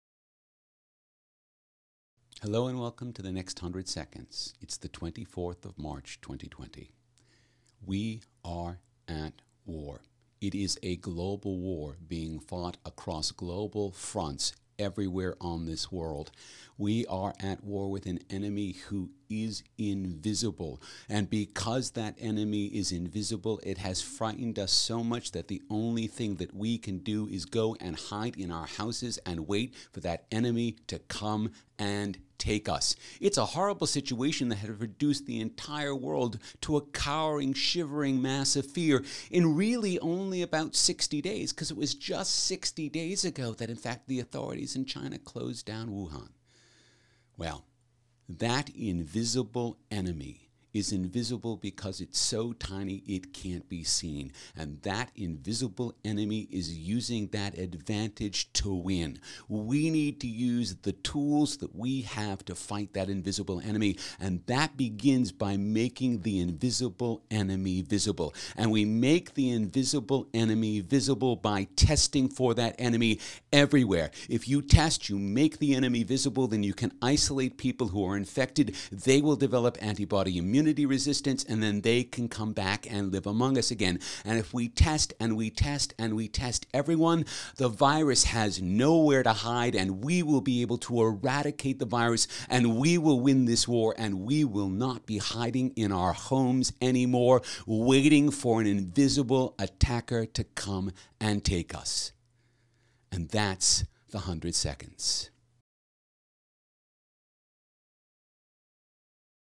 THE NEXT HUNDRED SECONDS // 24 MARCH 2020
We are at war.
An invisible enemy has invaded the world and driven us to hide in our homes.

But we have a weapon.
We can make the invisible visible.
And we can win.

Please share wherever it might help.